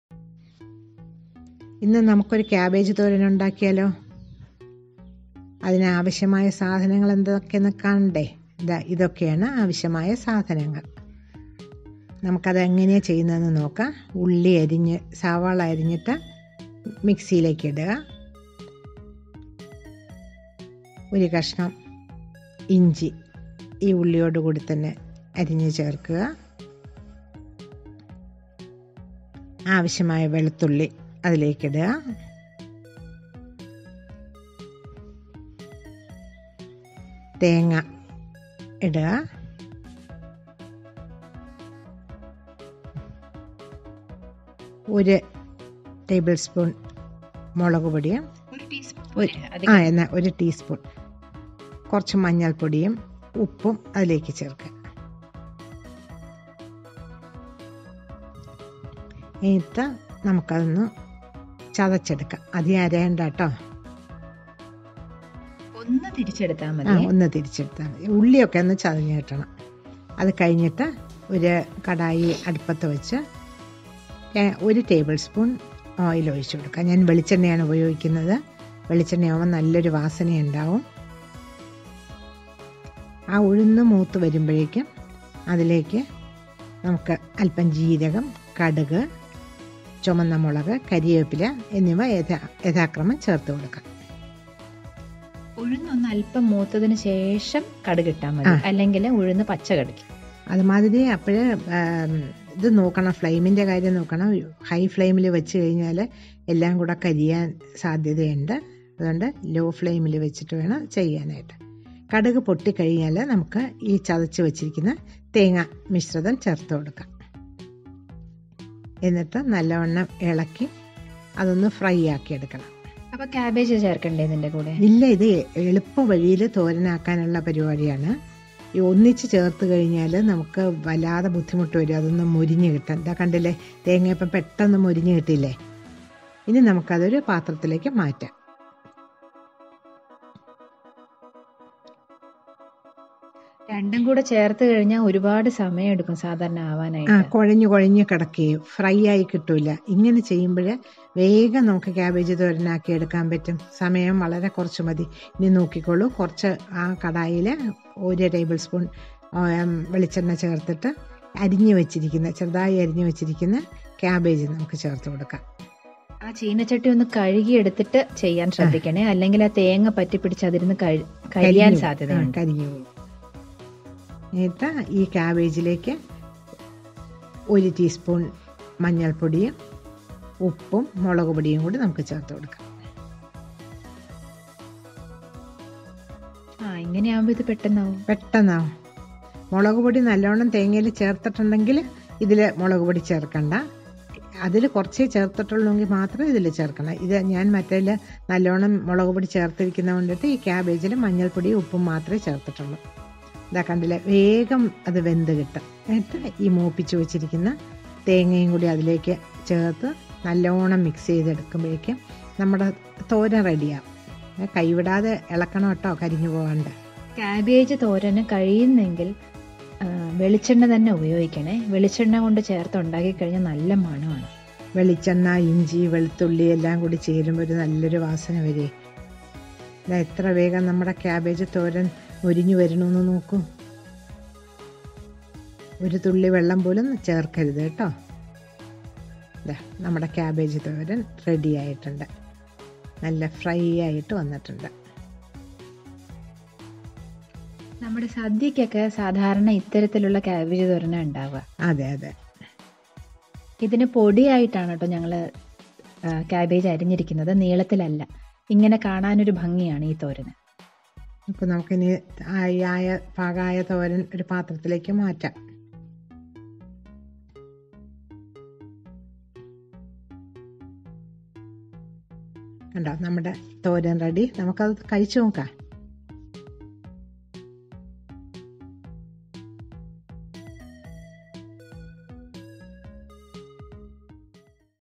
Cabbagethoran മലയാളം- Cabbage thoran/ Cabbage coconut stir fry/ Kerala style cabbage thoran
Thoran is a dry vegetable stir fry/curry from Kerala prepared with any fresh vegetables. This is a very easy and quick stir fry which goes well with rice.  Cabbage Thoran is one of the many dishes in the Onam Sadya menu.
Ingredients
Finely chopped cabbage- 2 cups
Coconut grated- 1 cup
Onion- 1
Dehusked black gram split- 1 tsp
Dried red chilly whole- 1